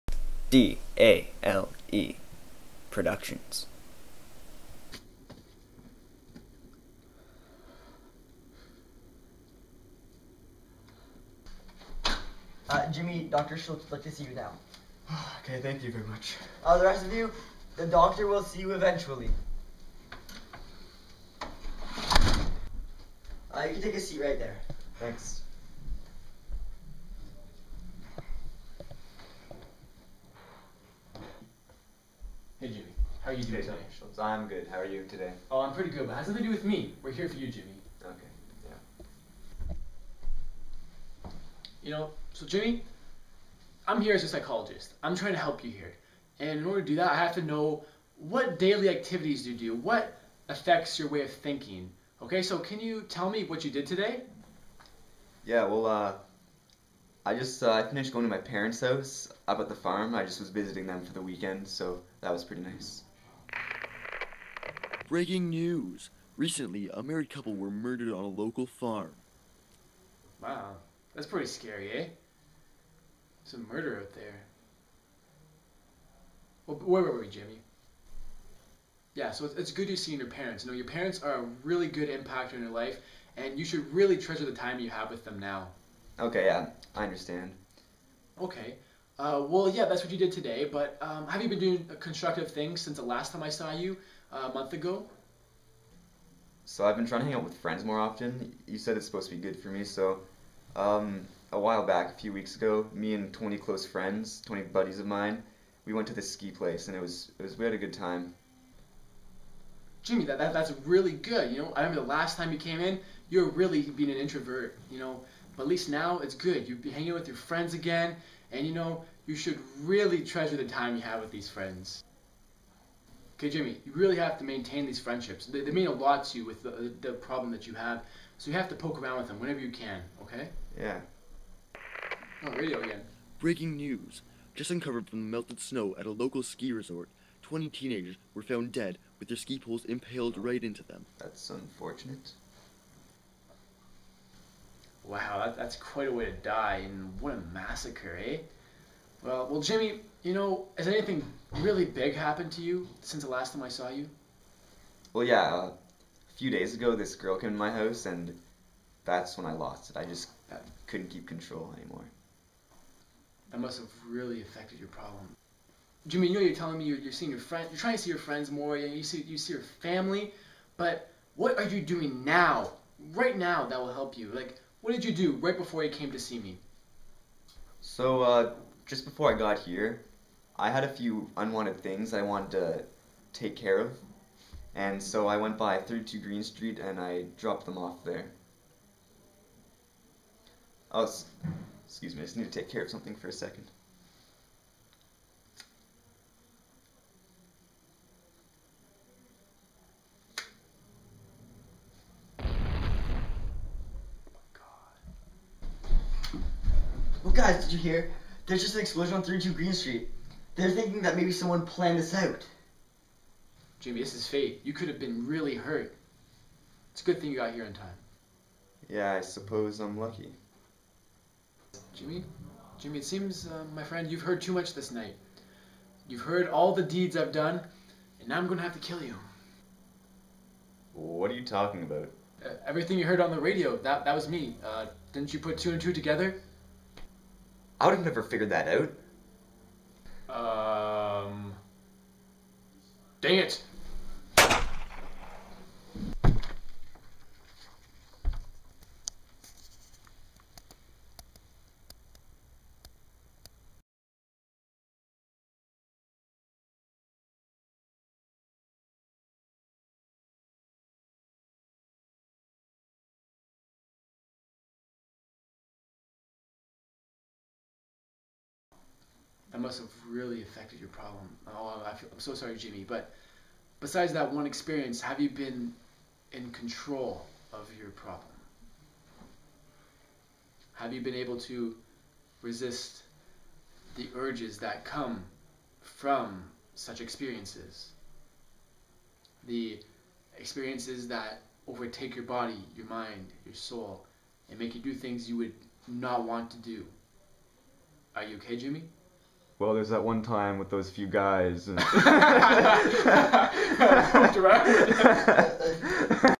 DALE Psychologist
Watch what happens when world renowned psychologist, Dr. Schultz, tries to help Jimmy.

DALE Psychologist, a short video by DALE Productions.